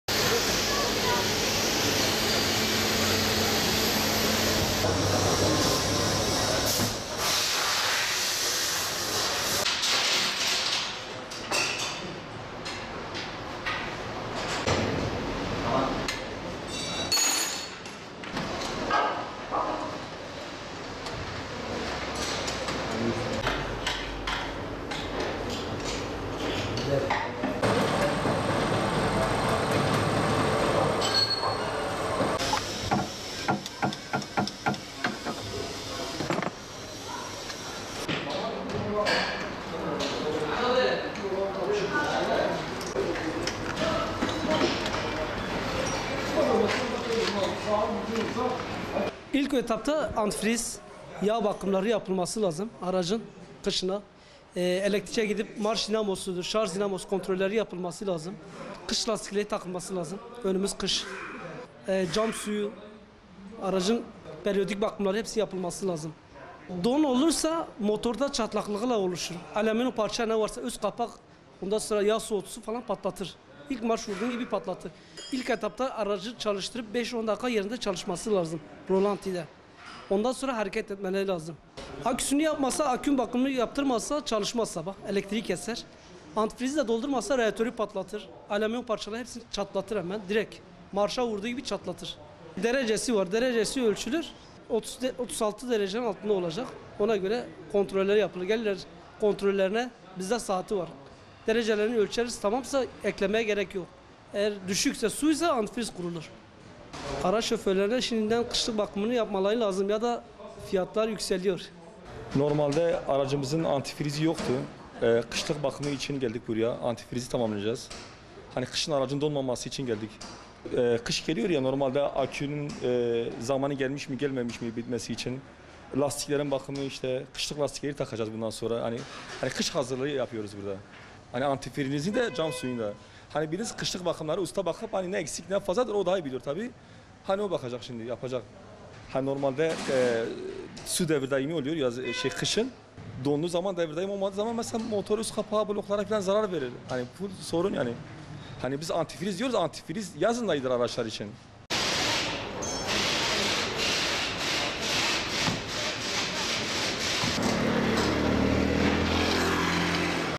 Oto tamircilerde “kış bakımı” mesaisi
Diyarbakır'da otomobil sahipleri, araçlarının kışlık bakımları için oto tamircilerinin yolunu tutarken uzmanlar ise sürücülerin antifriz kontrolü, kış lastikleri takmaları ve cam sularının kontrol edilmesi uyarısında bulunuyor.